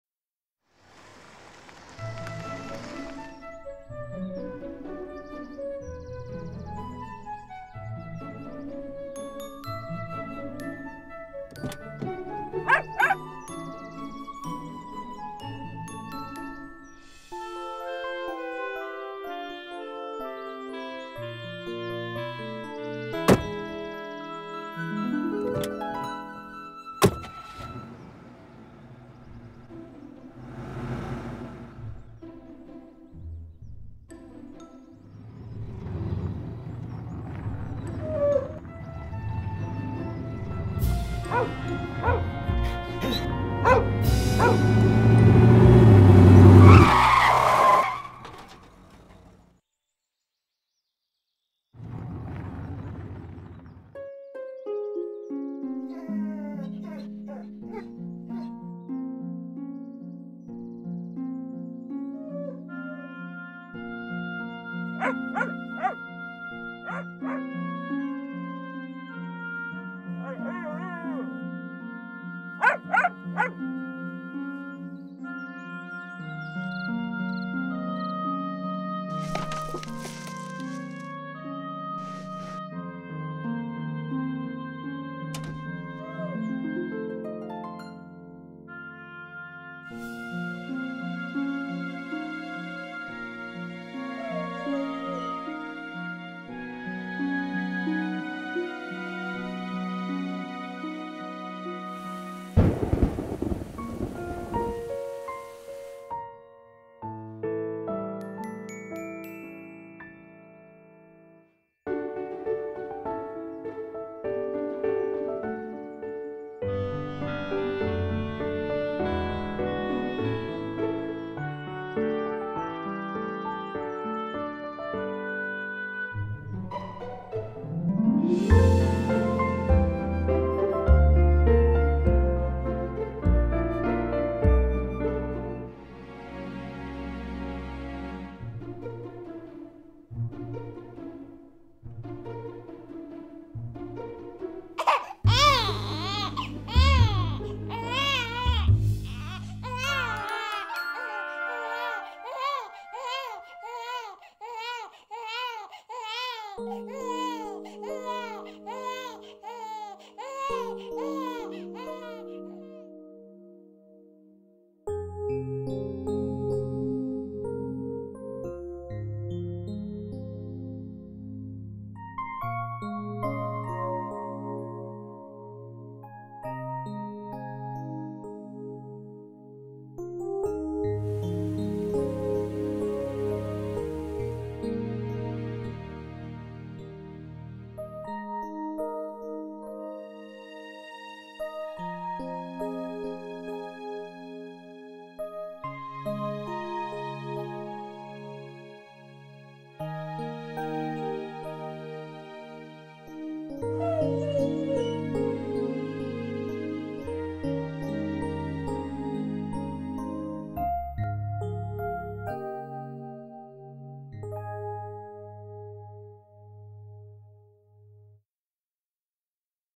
Do Not Abandon Your Pet
A dog suffers from hardship and emotional pain after his owner decided to abandon him at the park one day. "Do Not Abandon Your Pet" reminds you that you should never abandon your pet even if you have other commitments.